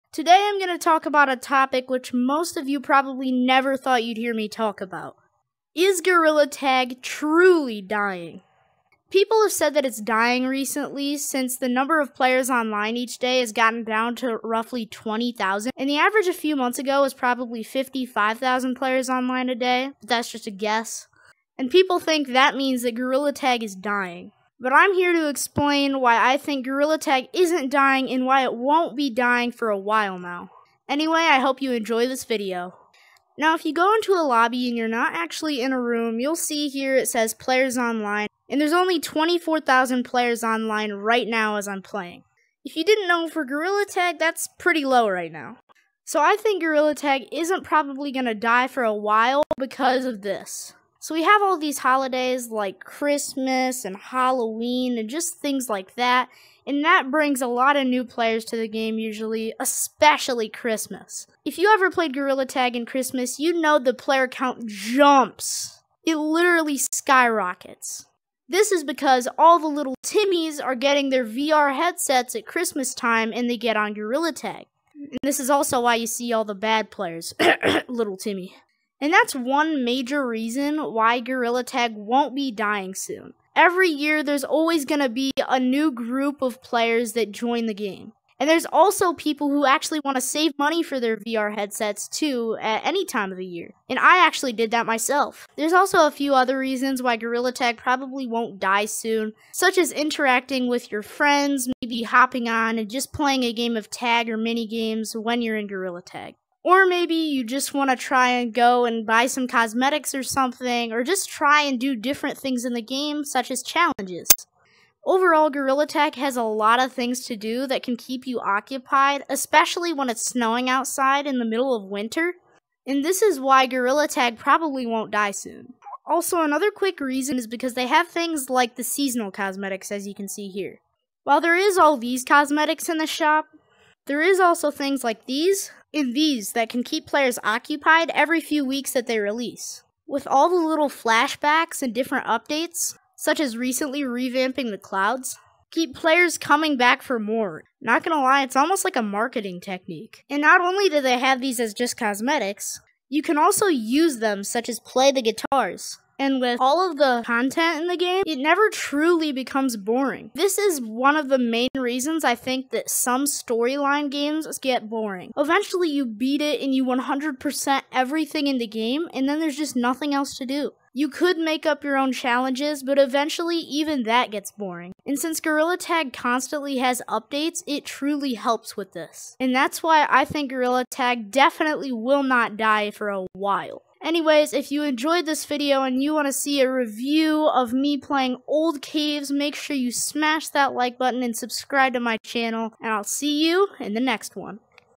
Is Gorilla Tag TRULY Dying?
Today, I talk about whether Gorilla Tag is actually dying, or not. People say it is, so I'm gonna prove them wrong! Enjoy the video guys! :)